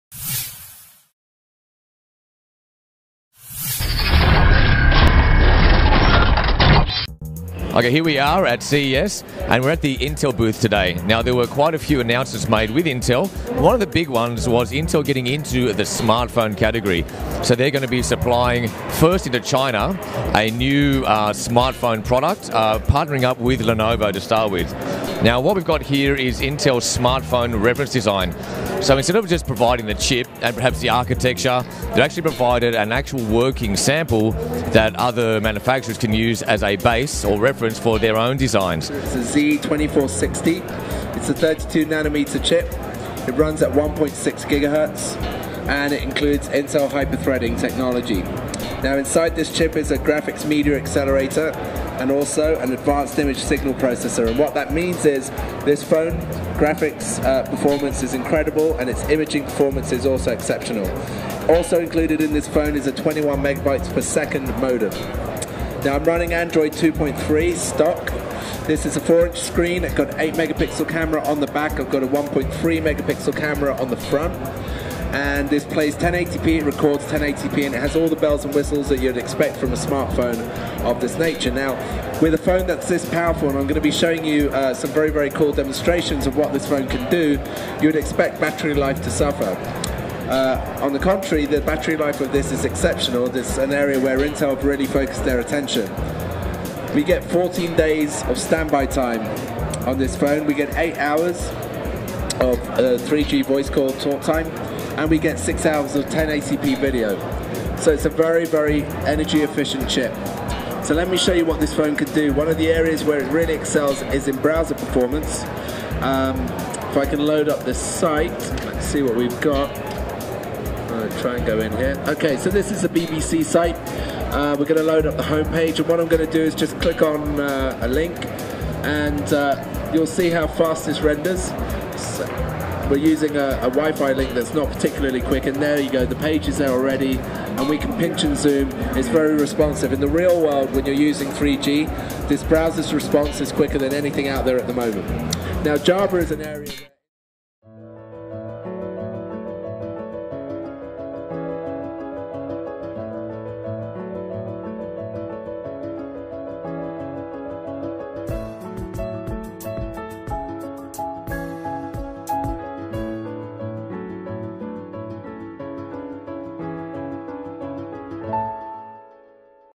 CES 2012 Intel Smartphone Part 1: Specs and Browsing
After watching the video click here for more CES 2012 coverage
A demonstration of the new Intel Reference Design smartphones, looking at the specs of this Intel-based smartphone and the browsing functionality.